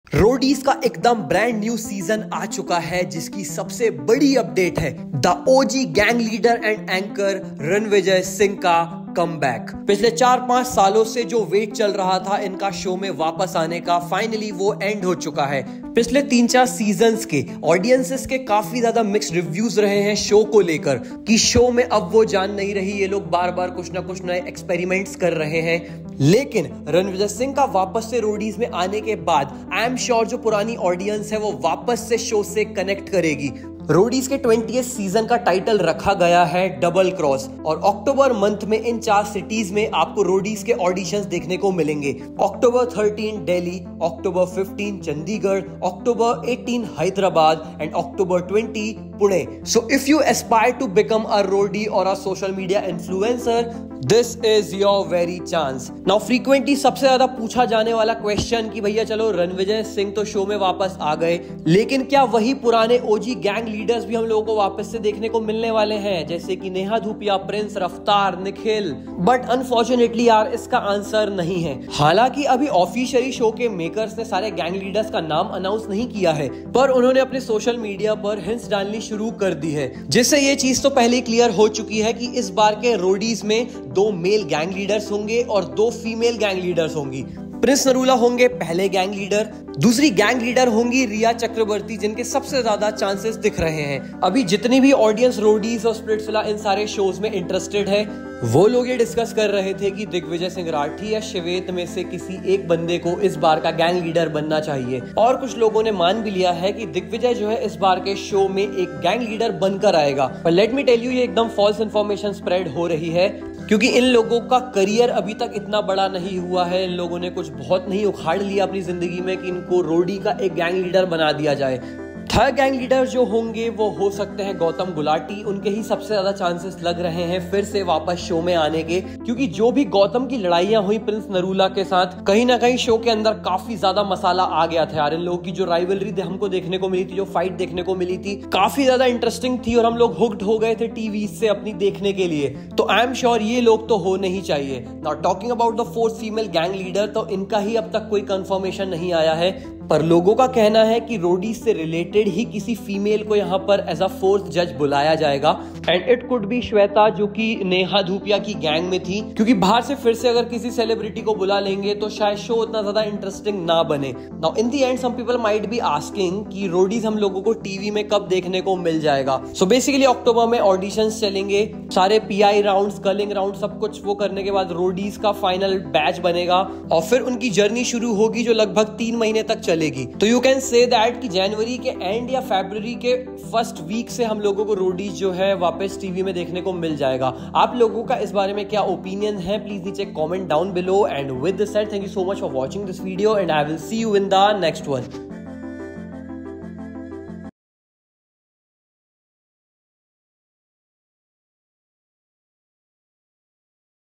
Roadies Double Cross All 4 New Gangleaders Revealed | Roadies New Season 2025 Judges Names
Rannvijay Singha has announced the audition dates for Roadies XX Double Cross, which will mark his return as an OG host after a four-year hiatus.
MTV Roadies XX: Double Cross Auditions: OG host Rannvijay Singha is all set to return after a four-year hiatus with the 20th season. The makers have announced the audition dates along with the cities where they will be conducting them. Read on to learn the details of the upcoming season.

Roadies Season 20: Know audition date and cities
On September 21 makers unveiled the teaser of Roadies season 20 announcing Rannvijay as a host. In the video, the actor makes a stylish entry on a motorbike. He then asks, "Roadie banega tu". The next frame shares the details of the auditions which will take place in Delhi, Chandigarh, Hyderabad and Pune.